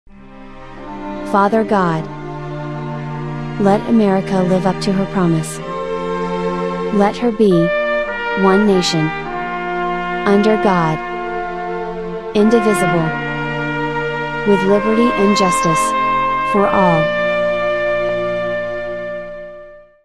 A prayer for the promise of America - FREE DOWNLOAD
A prayer for the promise of America

Father God!
Let America live up to her promise.
LET HER BE ONE NATION, UNDER GOD!
INDIVISIBLE
WITH LIBERTY AND JUSTICE
FOR ALL!!

Share this message on your church's social media platforms or download (link below) and use as you see fit.